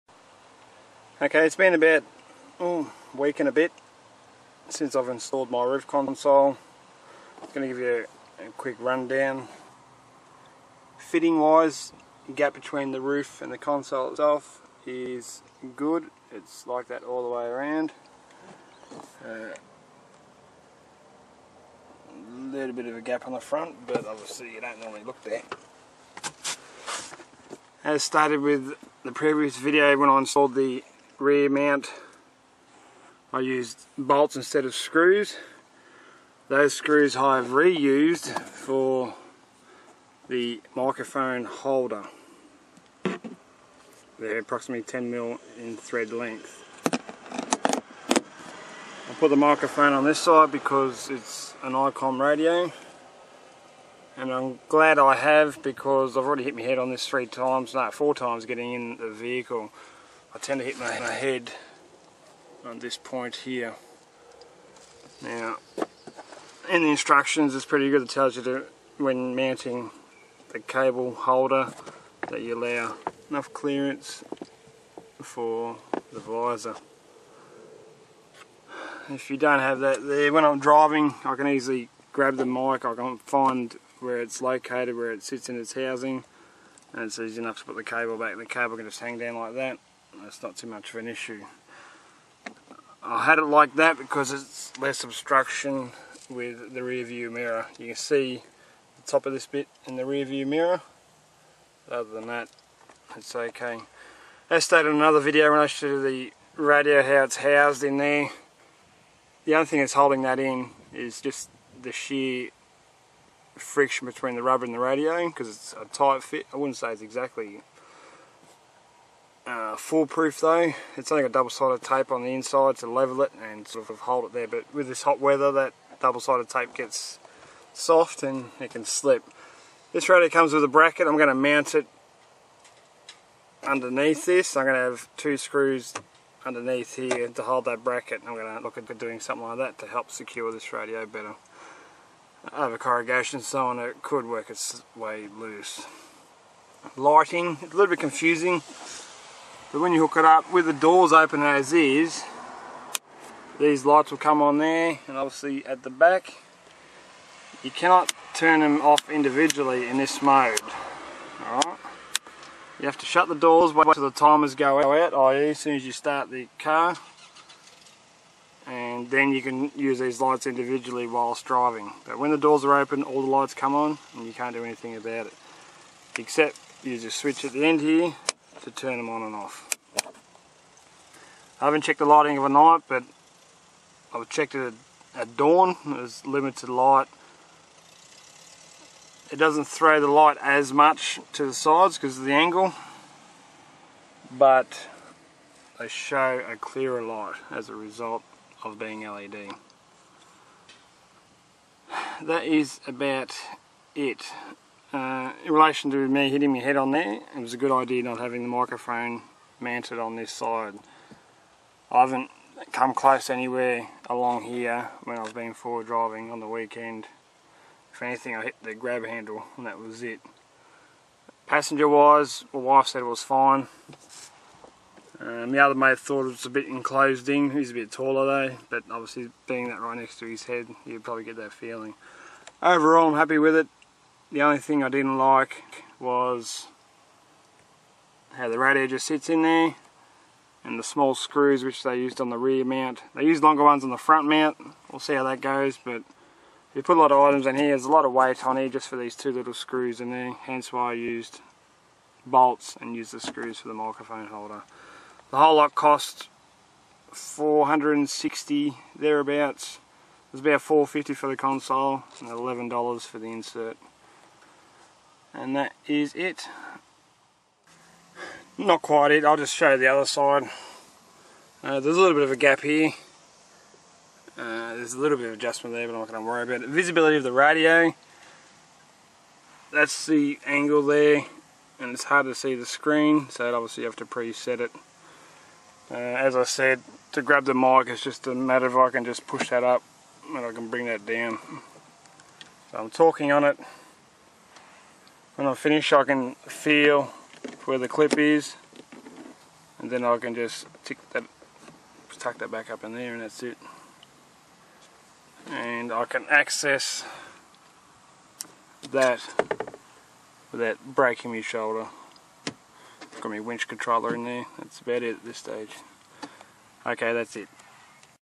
Outback Roof Console install for 120 series Prado Part 4 and final.mp4
Outback 4X4 Interior Roof Console install for a 2009 120 Series D4D manual Prado